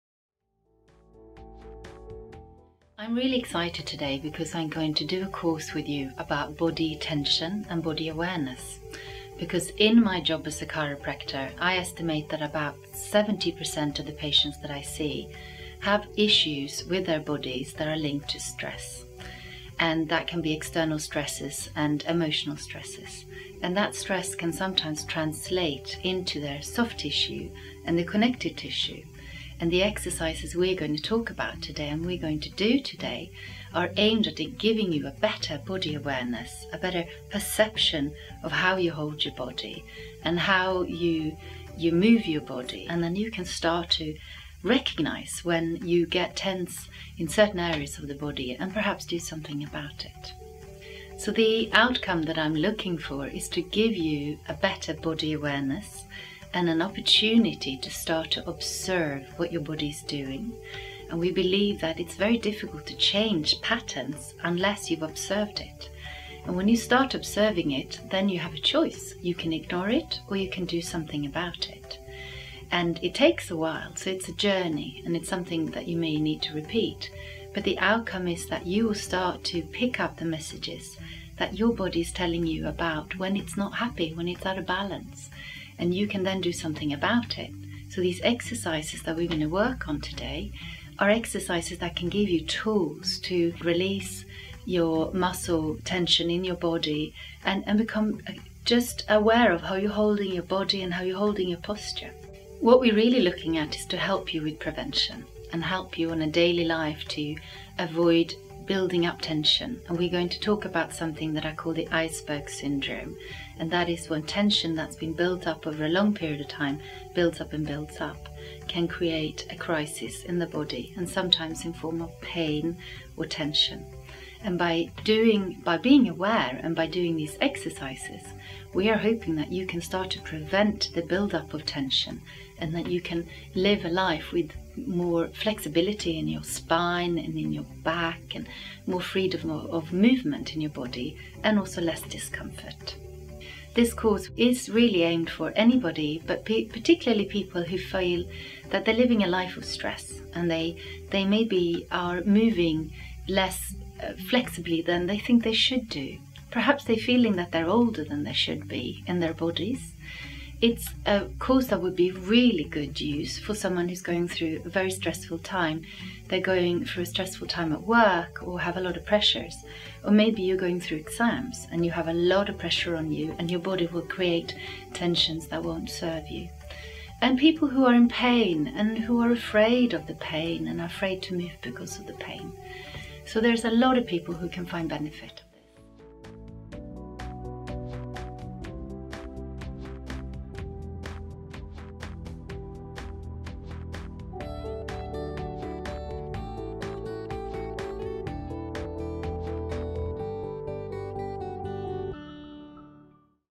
Mindful Body Movements- how to move more gently.
Mindful Body Movements is a new concept created by Chiropractor Tone Tellefsen Hughes after 25 years of working with patients. She works integrated with other therapists in her clinic helping patients find a better balance and alignment in their structure and posture. The course is aimed to help those who would like to start moving more and get connected with their bodies before starting an exercising programme. She draws on her experience of chiropractic, Alexander technique, yoga and Pilates and have created this fusion of movements to help those who need to find their way back to better confidence with their bodies. Here is the link to the course